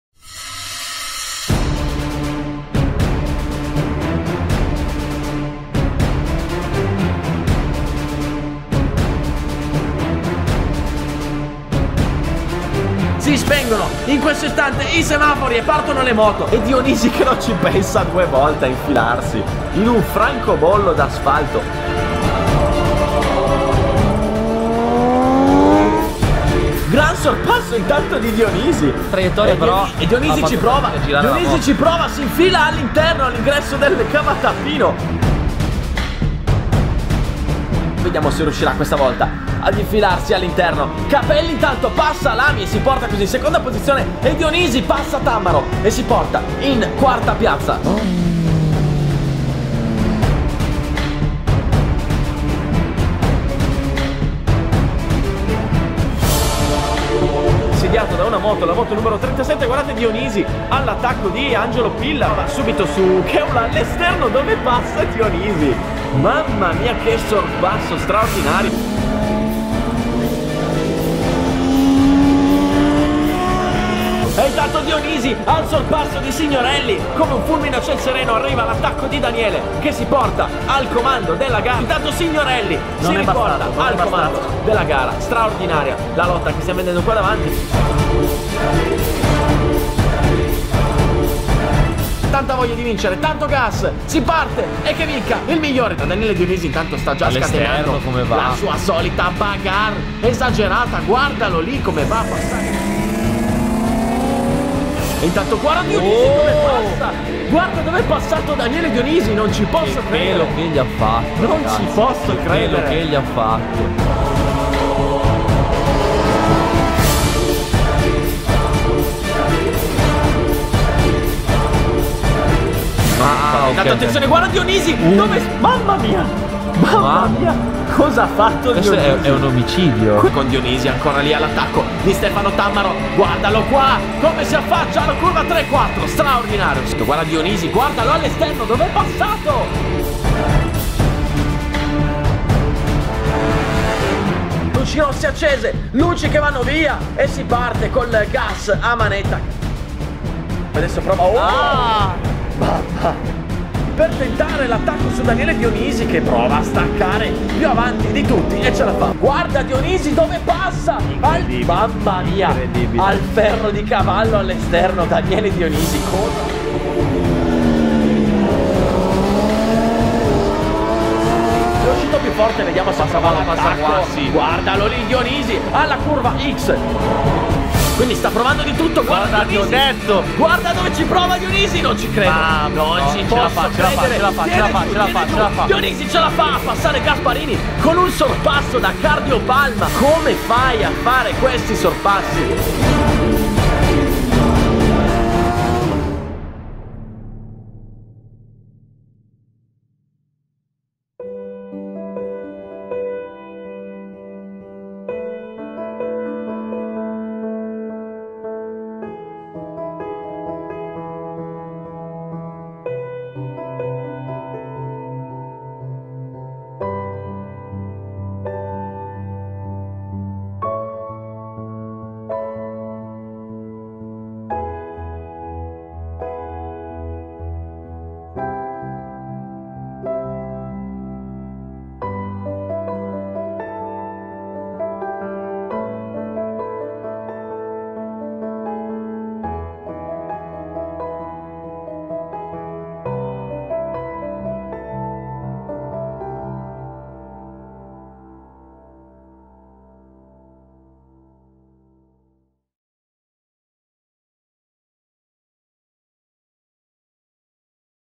Daniele Dionisi #3 - I migliori sorpassi del 2014 - RACE ATTACK 600
La straordinaria stagione di Daniele Dionisi, pilota della Race Attack, riassunta in questo video spettacolare! Tutti i suoi sorpassi più belli commentati da Alberto Fontana.
Da non perdere :)

Visitate per vedere altri video, vedere la classifica dei tempi su tutte le piste.